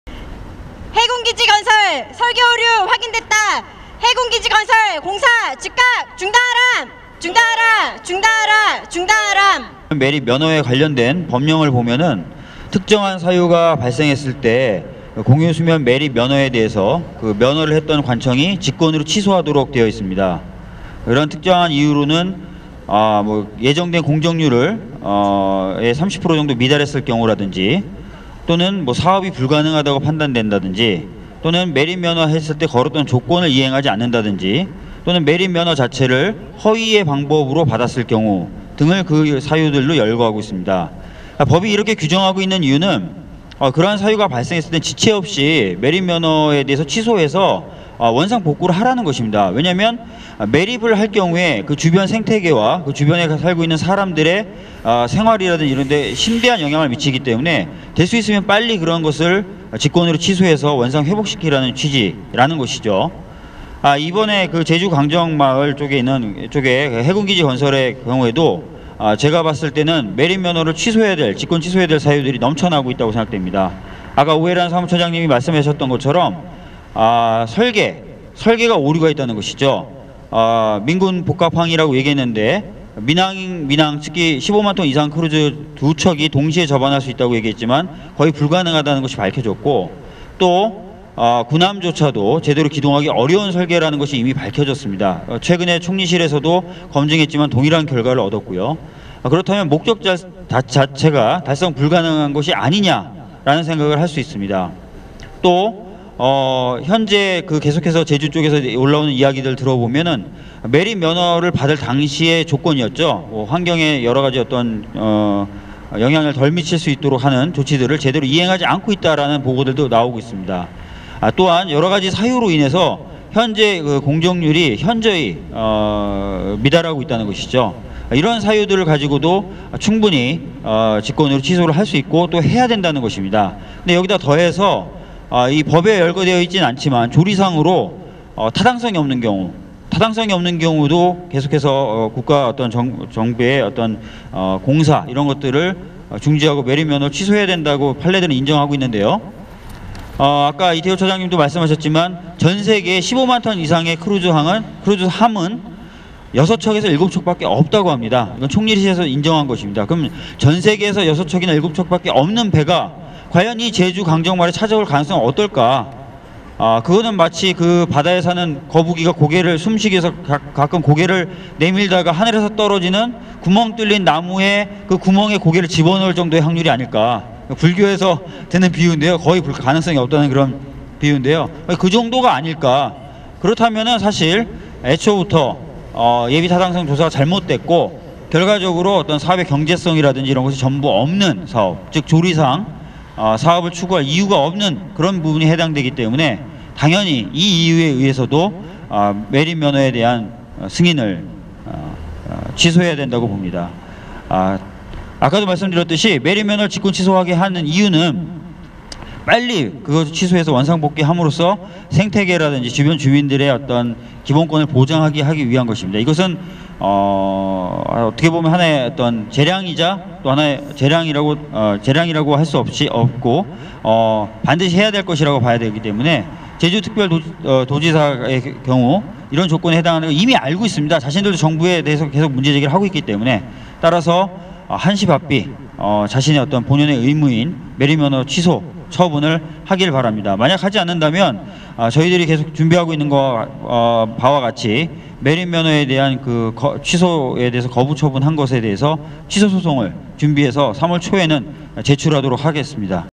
2012.2.23 제주해군기지건설 강행하는 총리실 규탄 긴급기자회견 3
[ 제주해군기지건설 저지를 위한 전국대책회의 긴급기자회견 ]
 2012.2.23 오후 5시 서울 정부종합청사 앞

제주해군기지 설계오류 검증보고서 결론조작, 총리실을 규탄한다.
- 불법공사 강행모의, 관계기관 대책회의 즉각 중단하라!

* 규탄발언 : 박주민 (변호사)